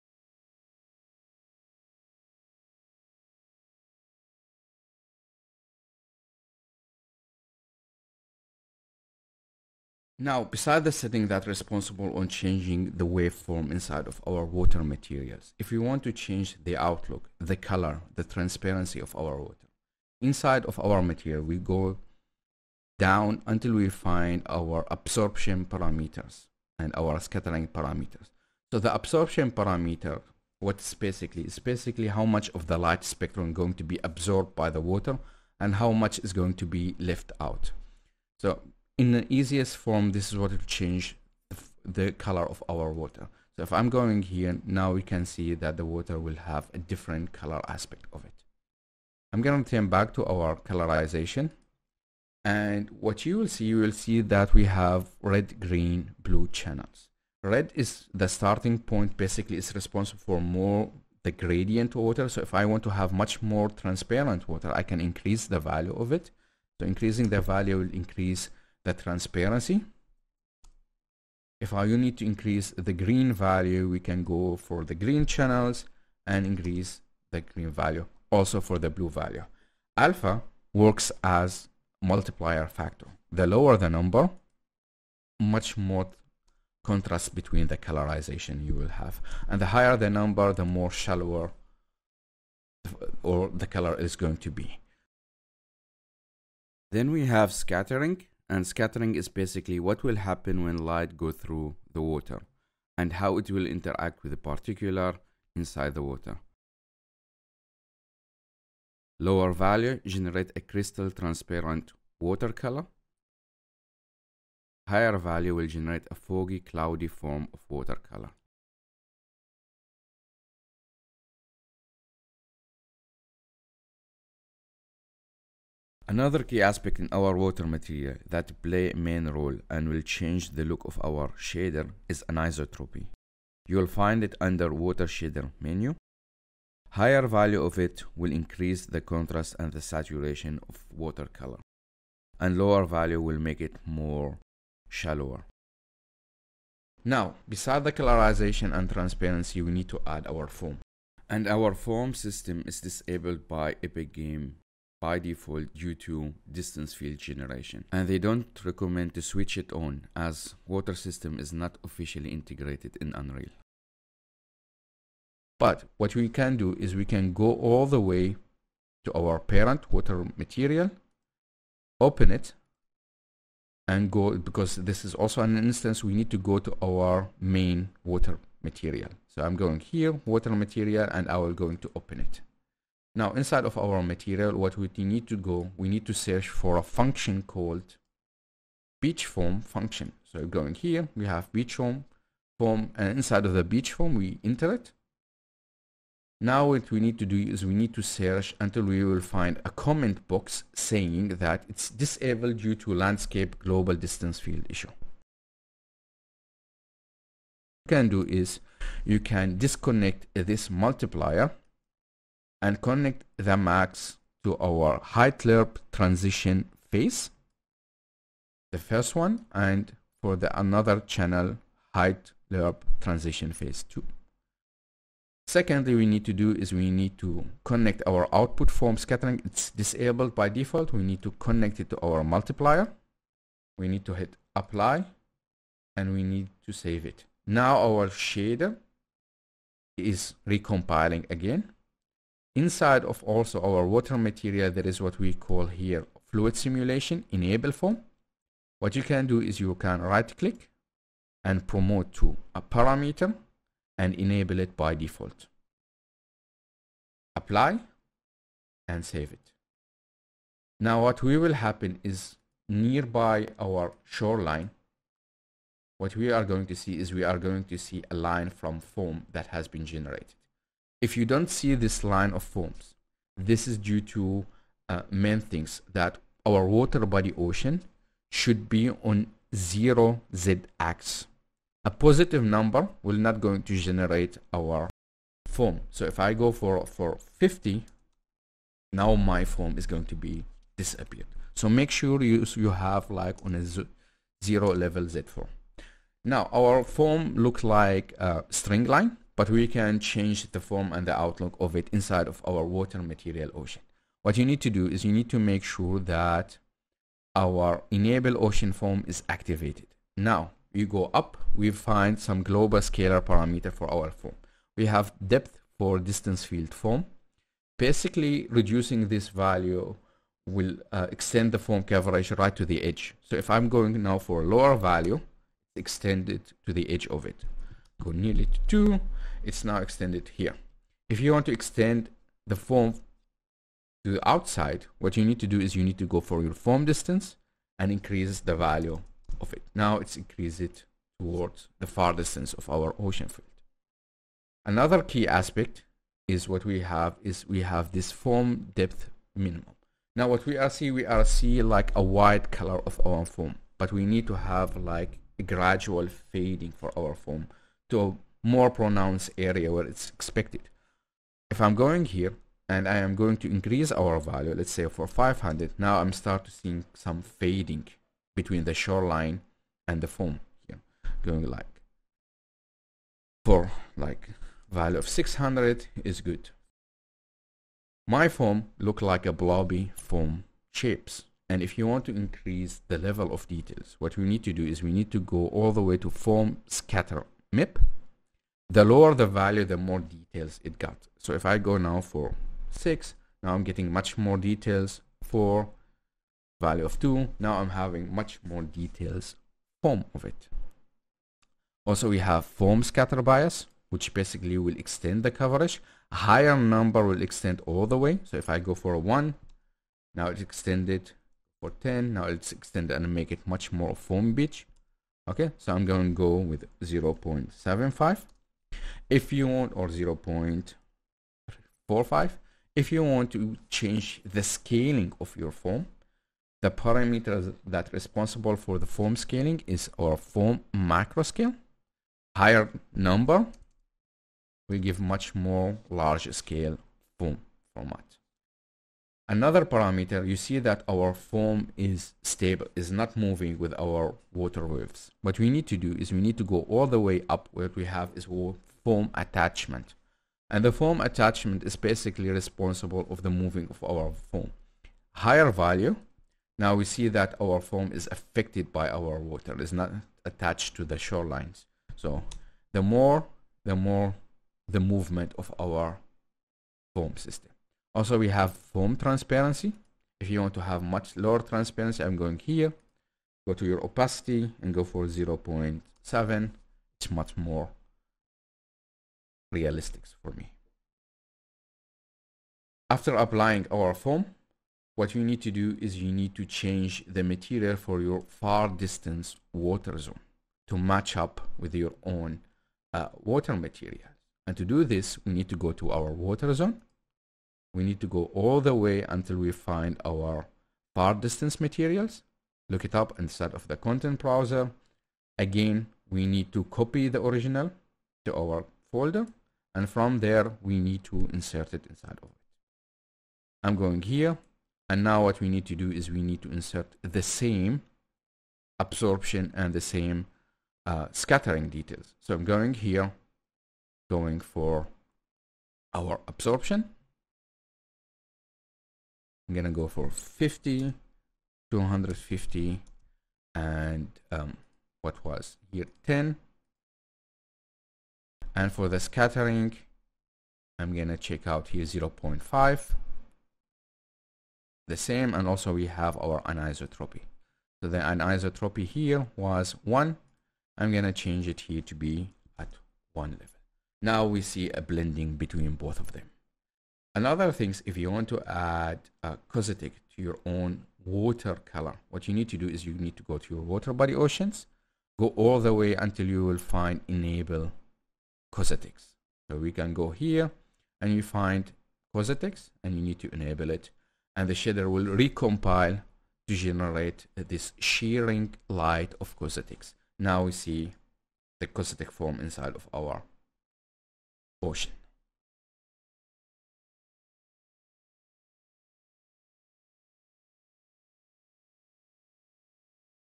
UE5 Water Material Tutorial: Color, Transparency, Foam, and Caustics
Description

Today, Mahmoud Alkawadri will show you how to understand and manipulate the water material system in Unreal Engine 5. In this comprehensive lesson, you’ll learn how to adjust the absorption and scattering parameters to control the color and transparency of your water, creating stunning visual effects. Discover how to tweak the anisotropy settings to enhance the contrast and saturation of your water shader for a more vibrant look.

Mahmoud will guide you through the process of enabling and customizing foam effects, even though Epic Games disables this by default due to distance field generation issues. You’ll learn step-by-step how to modify the beach foam function, adjust parameters for a more natural shoreline appearance, and ensure that foam integrates seamlessly with your water waves.

Explore advanced settings such as foam depth, foam scatter MIP, and foam transparency to refine the detail and realism of your water materials. Additionally, Mahmoud will demonstrate how to add caustic effects to give your water an extra layer of depth and realism.

Want to master these techniques and much more? Head to CGCircuit to access our full tutorial!